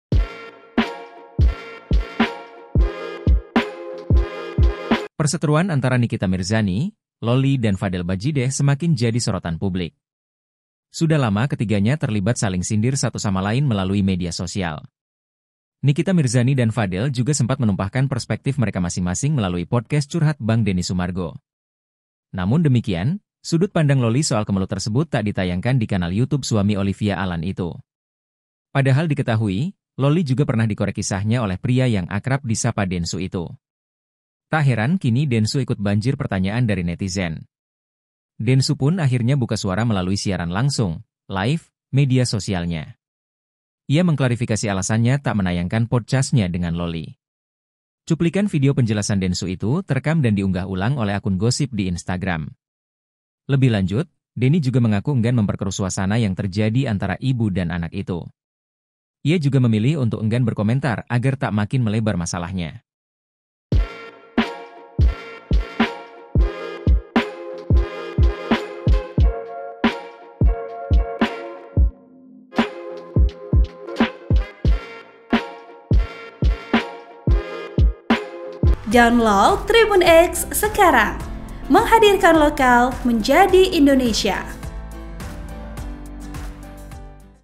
Denny Sumargo Akhirnya Beber Pemicu Tak Tayangkan Podcast Bareng Lolly, Begini Klarifikasi Densu
TRIBUN-VIDEO.COM  - Perseteruan antara Nikita Mirzani, Lolly dan Vadel Badjideh semakin jadi sorotan publik.

Nikita Mirzani dan Vadel juga sempat menumpahkan perspektif mereka masing-masing melalui podcast Curhat Bang Denny Sumargo.

Namun demikian, sudut pandang Lolly soal kemelut tersebut tak ditayangkan di kanal Youtube suami Olivia Allan itu.

Padahal diketahui, Lolly juga pernah dikorek kisahnya oleh pria yang akrab disapa Densu itu.

Tak heran kini Densu ikut banjir pertanyaan dari netizen.

Densu pun akhirnya buka suara melalui siaran langsung (live) media sosialnya.

Ia mengklarifikasi alasannya tak menayangkan podcastnya dengan Lolly.

Cuplikan video penjelasan Densu itu terekam dan diunggah ulang oleh akun gosip di Instagram.

"Bang Densu, kenapa podcast Lolly nggak tayang.

Waduh, saya kalau cerita nggak enak nanti kalau ribut-ribut ya.

Sudah ya, tidak perlu diceritakan. Kadang-kadang ada hal yang lebih baik kita cari baiknya," ucap Densu dikutip via Grid.id, Senin (23/9/2024).

Lebih lanjut, Denny juga mengaku enggan memperkeruh suasana yang terjadi antara ibu dan anak itu.

Ia juga memilih untuk enggan berkomentar agar tak makin melebar masalahnya.

"Daripada nanti ribut-ribut tak bagus. Soalnya ibu anak ribut-ribut tidak bagus dilihat.

Jadi saya memilih untuk tidak berkomentar.

Tidak bagus, kalau menjelaskan, tidak perlu menjelaskan. Takutnya nanti ribut-ribut," imbuh Densu.

Densu juga menegaskan ia tak menyesal sempat menayangkan podcast dari Vadel Badjideh yakni kekasih dari Lolly.

"Nggak, karena saya menghargai setiap orang seperti manusia. Mau dia salah, mau dia salah itu kan urusan dia.

Saya tidak pernah diajarkan untuk menginjak siapapun, meremehkan siapapun.

Sehina apapun hidupnya, saya ajarkan setiap agama tidak mengajarkan seperti itu.

Itu kan hidup dia, saya tidak perlu ikuti hidup dia, saya tidak boleh merendahkan orang itu. Saya tidak suka orang begitu," beber Denny Sumargo.

Terakhir, Densu mengatakan bahwa podcast miliknya bukanlah tempat untuk menghakimi seseorang.

Melainkan hanya wadah yang disediakannya bagi siapapun yang ingin berbagi cerita.

"Meja podcast saya itu bukan meja hakim. Tempat podcast saya adalah tempat orang bercerita," kata suami dari Olivia Allan itu.

Soal orang yang bercerita jujur atau tidak, Densu mengembalikannya ke orang tersebut dan hanya Tuhan yang tahu.

"Mau ceritanya jujur, tidak jujur, mau nanti dia bohong, tidak bohong, itu kembali ke orangnya.

Masalah dia benar atau tidak yang tahu hanya Tuhan sama dia.

Setiap orang menanggung resiko daripada apa yang dia katakan karena Tuhan Maha Tahu.

Tuhan akan membantu orang-orang terzalimi dan Tuhan akan menunjukkan keburukan orang-orang yang berpura-pura baik," tandas Densu.

Artikel ini telah tayang di BanjarmasinPost.co.id dengan judul Cuma Berujung Jadi Arsip, Denny Sumargo Akhirnya Beber Pemicu Tak Tayangkan Podcast Bareng Lolly,

Editor Video: Riko Pulanggeni
Denny Sumargo, Pemicu, Tayangkan, Podcast Bareng, Lolly, Klarifikasi Densu